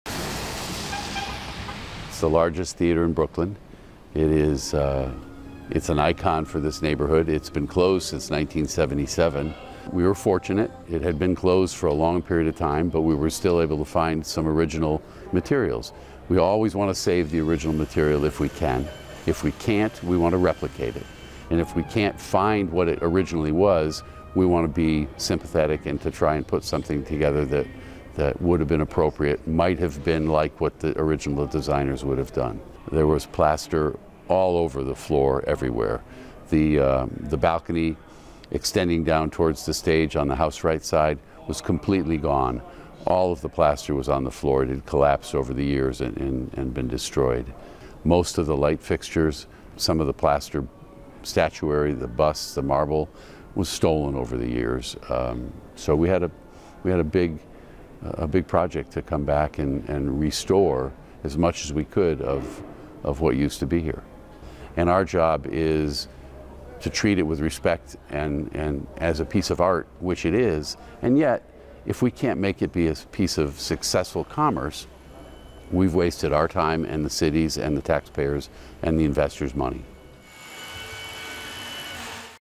A 1920's Brooklyn Theater Reborn
A once gilded Brooklyn movie palace that's been crumbling for decades, is back after a two-year, $95 million renovation. The theater that first opened weeks before the Wall Street crash is now the largest in New York's biggest borough. (Feb. 03)